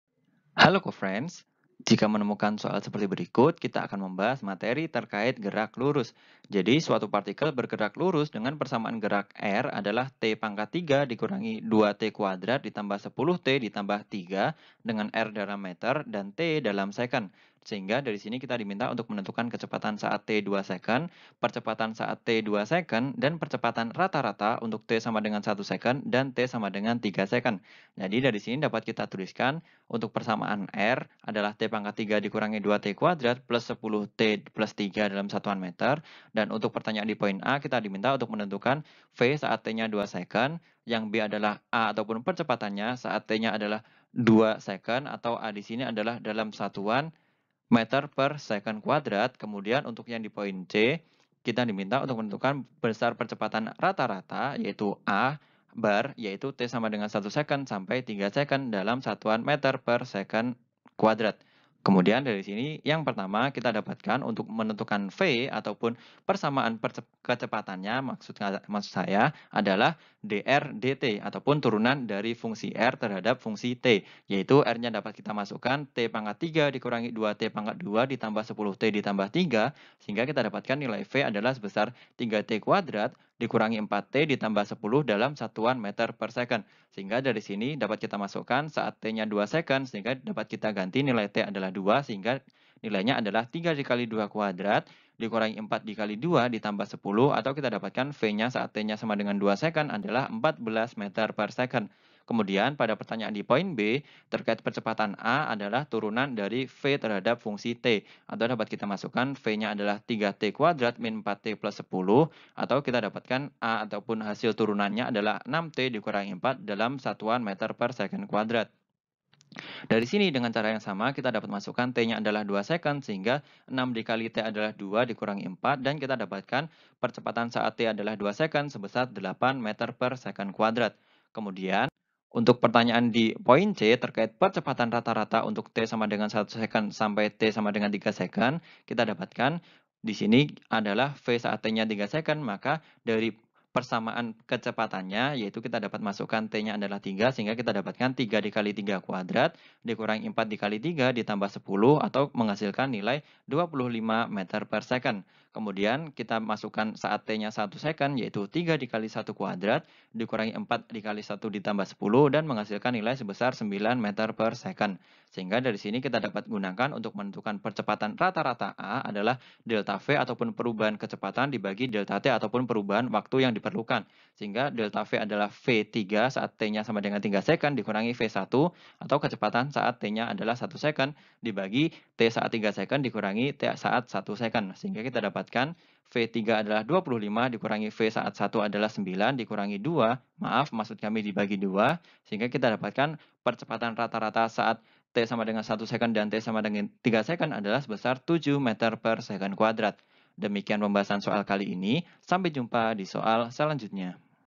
Suatu partikel bergerak lurus dengan persamaan 2t2 1Ot 3, gerak r =t3 dalam meter dan dalam sekon...
Suatu partikel bergerak lurus dengan persamaan 2t2 1Ot 3, gerak r =t3 dalam meter dan dalam sekon. Tentukan: r t kecepatan saat 2 sekon, a. t = 2 sekon, b. percepatan saat t = untuk t = 1 s dan t = 3 sl C. percepatan rata-rata

Topik atau Materi: Gerak Lurus dengan Kecepatan Konstan - Gerak Lurus - Mekanika - Fisika Kelas 10 SMA -

Sumber Soal: Fisika SMA Kelas 11, BH, 2009 , Pusat Perbukuan Departemen Pendidikan Nasional

00:00 : Bedah Soal
01:00 : Konsep, rumus dan pengertian Gerak Lurus dengan Kecepatan Konstan
01:18 : Langkah penyelesaian soal
02:24 : Jawaban akhir
02:54 : Penutup